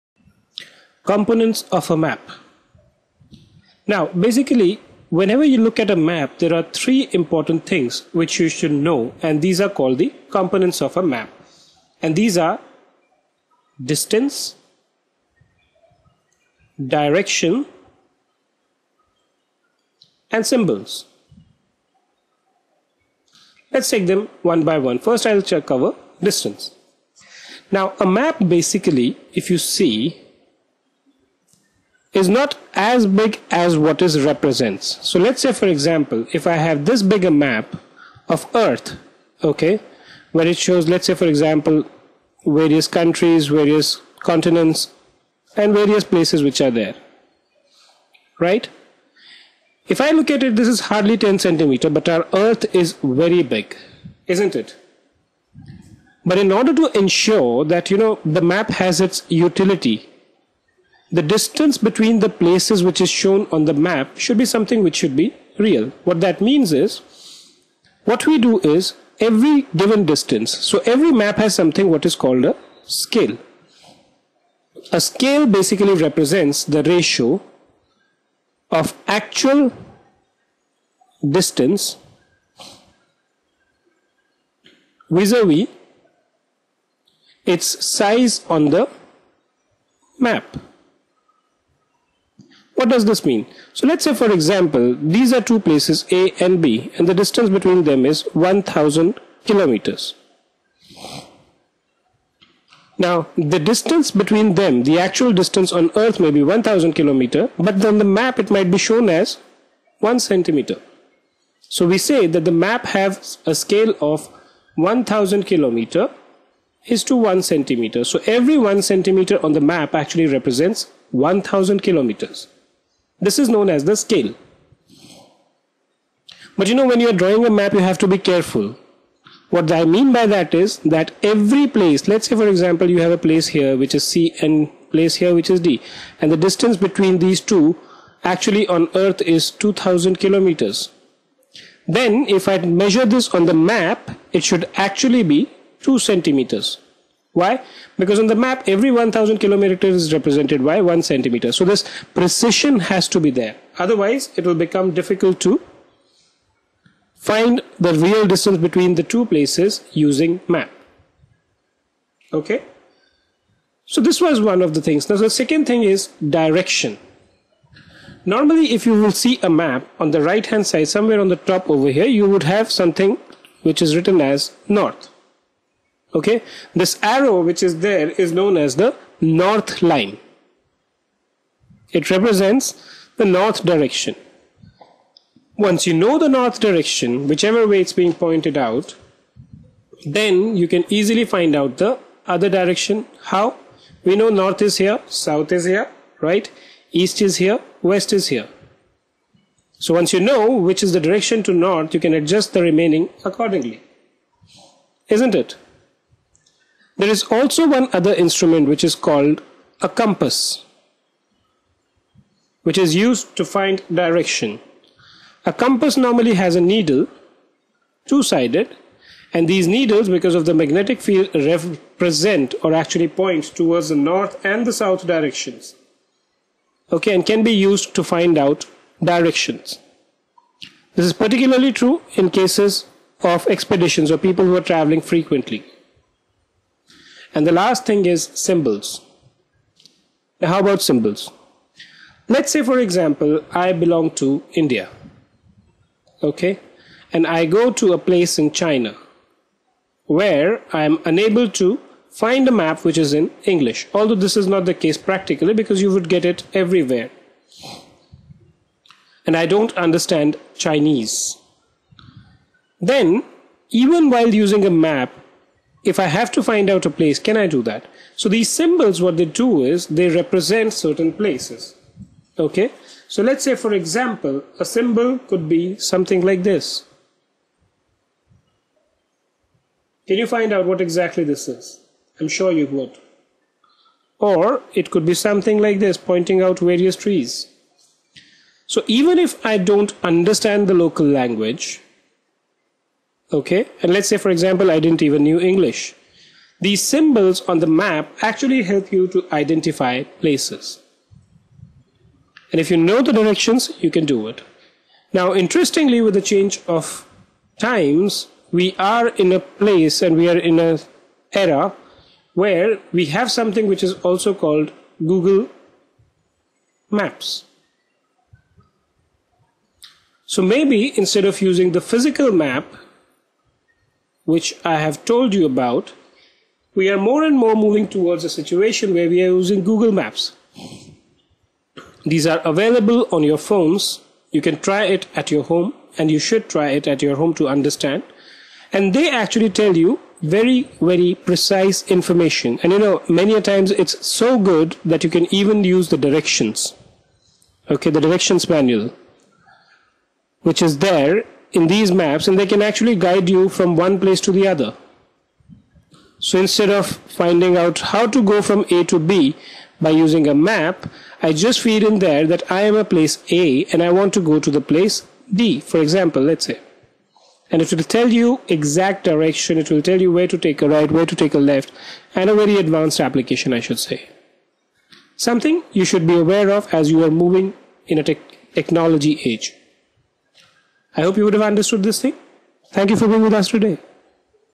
What are the three components of a Map - NCERT Geography Class 6
This video gives us an idea about What are the three components of a Map.
The three main components of a map are:
1) distance, which can be calculated by estimating the scale of the map. The distance of a map which is represented by a scale helps us understand how big or small the area is.
2) Compass Direction: A map contains latitudes and longitudes, which are imaginary lines to locate any point on the earth. Some maps may only contain a North arrow to show the Direction of North.
3) Symbols (or Key): Many maps are represented either by colours or symbols because it may look extremely clustered to put in all details on the map itself. Therefore, in the bottom left or right corner, there are legends to explain what a particular colour or symbol means. For example, Yellow depicts desert area, Green depicts plain land, Brown depicts hilly region. "

"Our next video topic - Sketch Map and a Plan - NCERT Geography Class 6